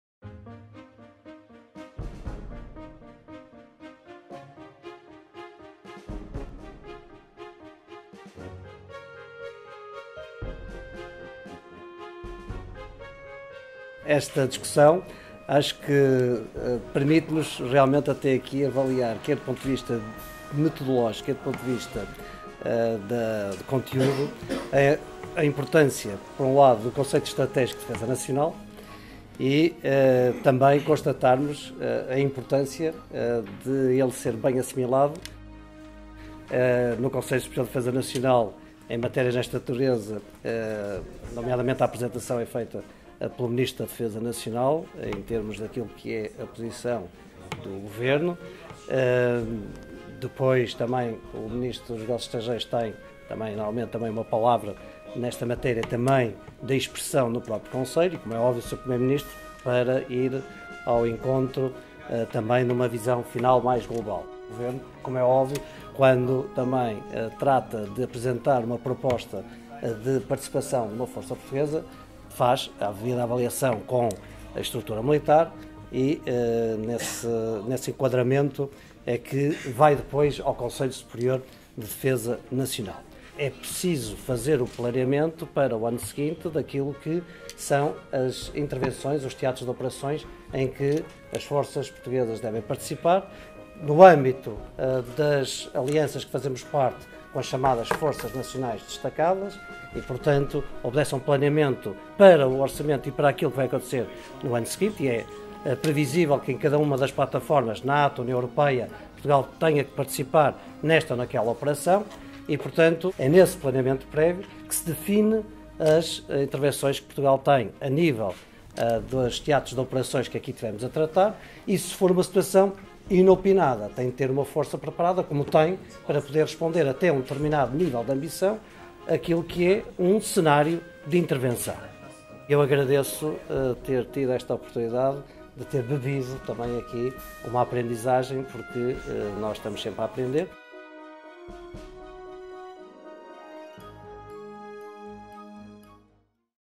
Simulação de um Conselho Superior de Defesa Nacional
O ministro da Defesa Nacional, José Pedro Aguiar-Branco, assistiu a uma simulação do Conselho Superior de Defesa Nacional, feita por estudantes da Universidade Lusíada, em Lisboa, esta quinta-feira.